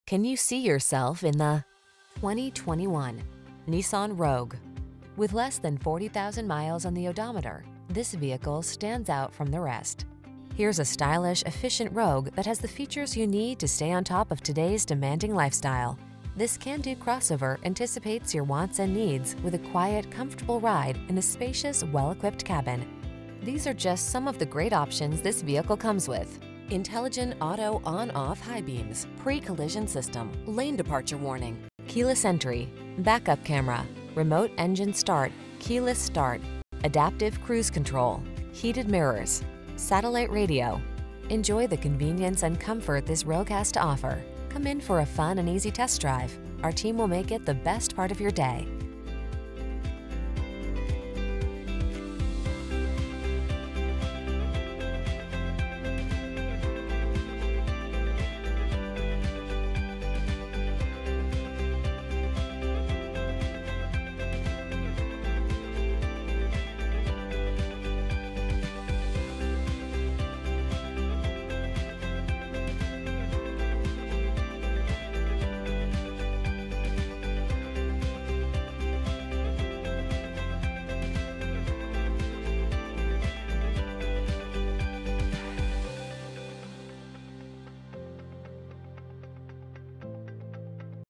2021 Nissan Rogue SV Ft. Pierce, Sebastian, Palm Bay, Melbourne FL
Pre-Owned 2021 Nissan Rogue SV available at Sutherlin Nissan Vero Beach located in 946 US-1, Vero Beach, FL, 32962. Servicing the Ft. Pierce, Sebastian, Palm Bay, Melbourne area.

Can you see yourself in the  2021  Nissan Rogue.  Here's a stylish, efficient Rogue that has the features you need to stay on top of today's demanding lifestyle. This can-do crossover anticipates your wants and needs with a quiet, comfortable ride in a spacious well-equipped cabin.  These are just some of the great options this vehicle comes with: Intelligent Auto on/off High Beams, Pre-Collision System, Lane Departure Warning, Keyless Entry, Back-Up Camera, Remote Engine Start, Keyless Start, Adaptive Cruise Control, Heated Mirrors, Satellite Radio. Enjoy the convenience and comfort this Rogue has to offer. Come in for a fun and easy test drive. Our team will make it the best part of your day.